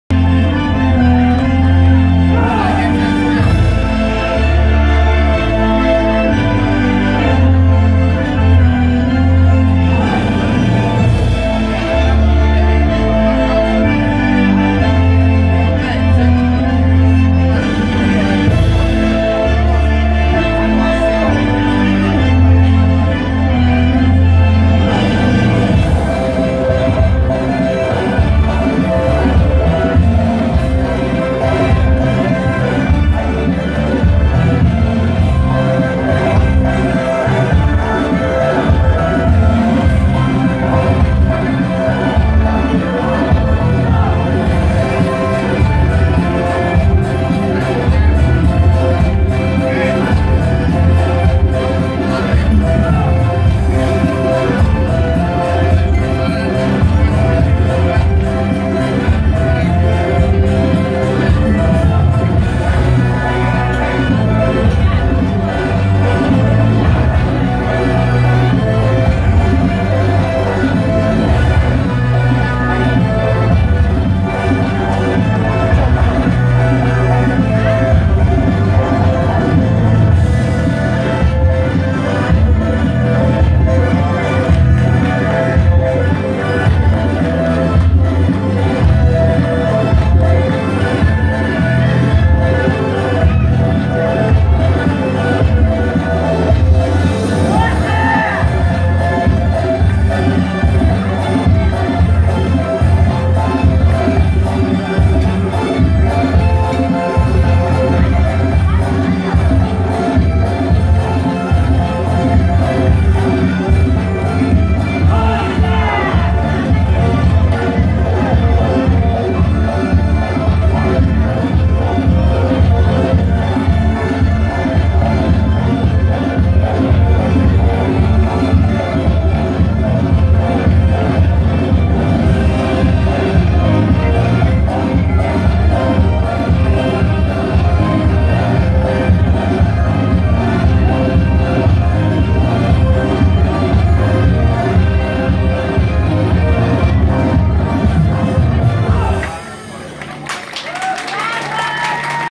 Beautiful Georgian Dances - Part II (Argo Cable Car/Folklore/Batumi)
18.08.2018 Georgian Folk Nights фольклор